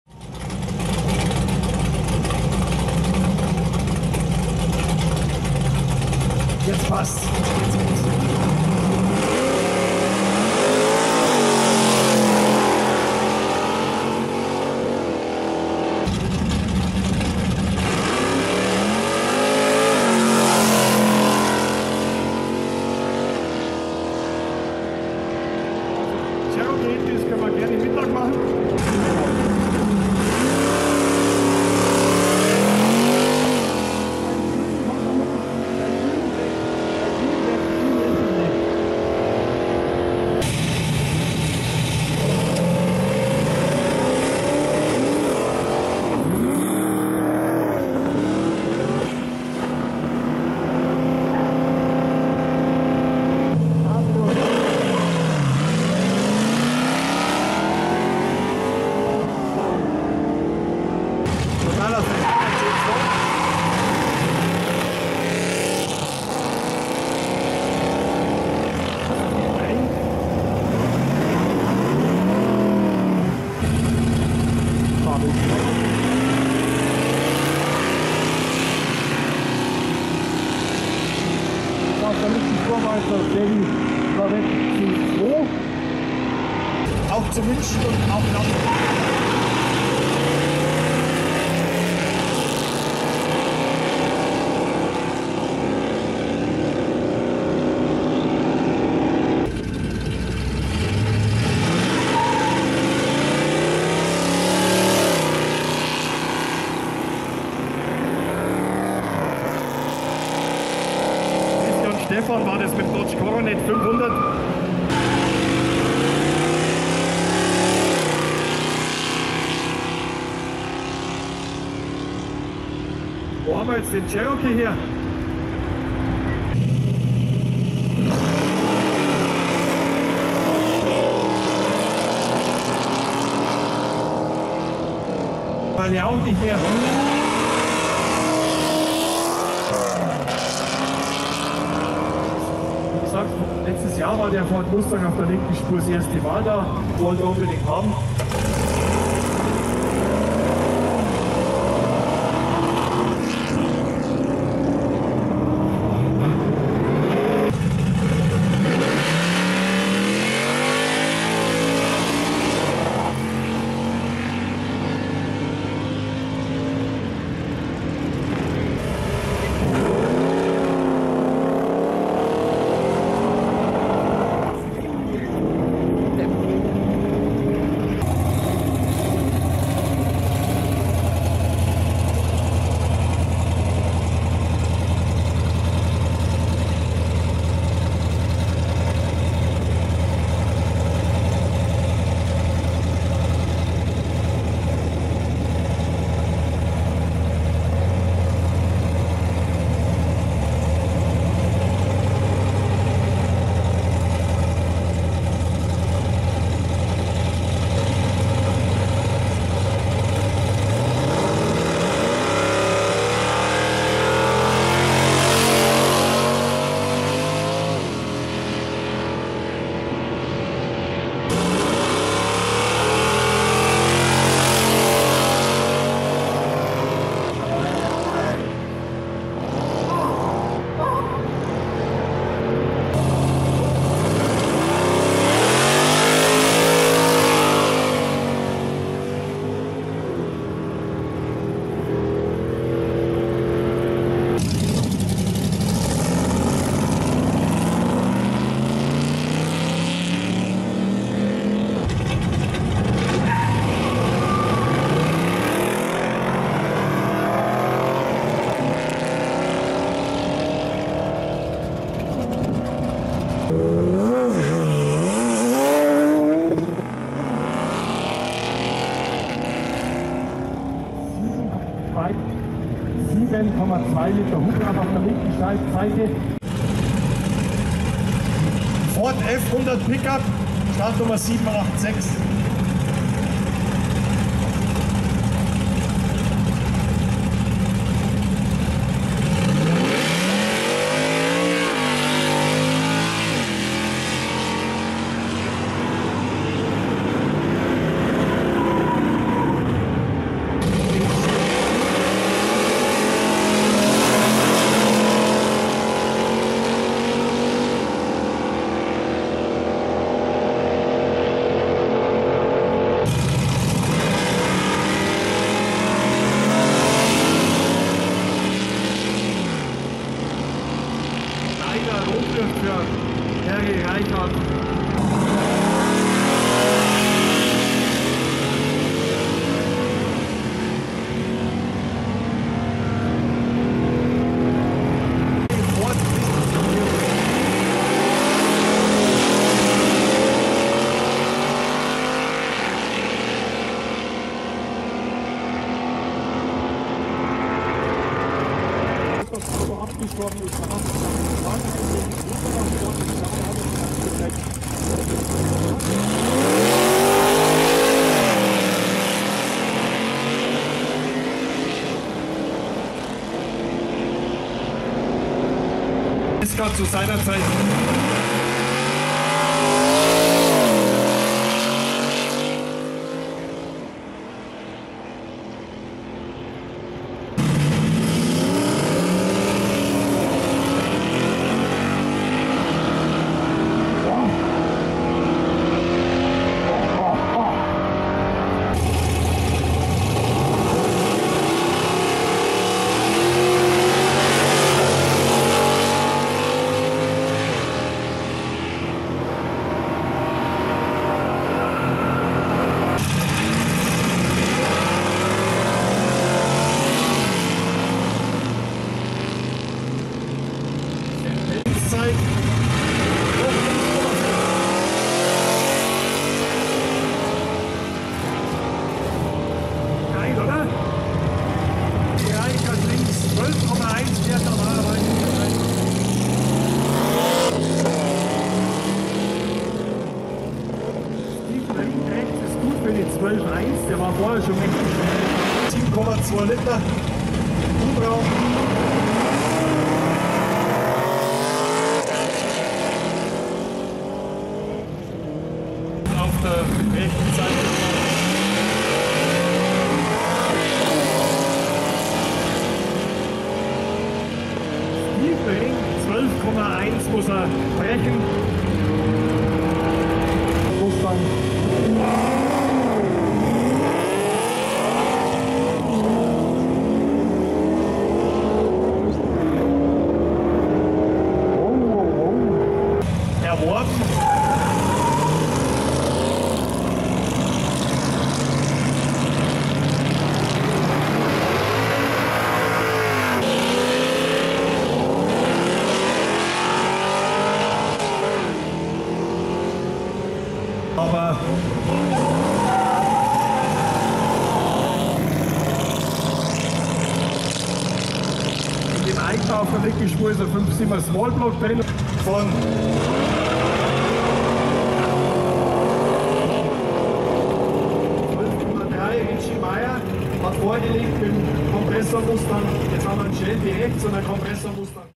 Race at Airport - Vilshofen 2018 - Klasse 7 - US-Cars
Mitschnitte vieler Läufe der Klasse 7.
Allerherzlichsten Dank dafür an Andrea!!!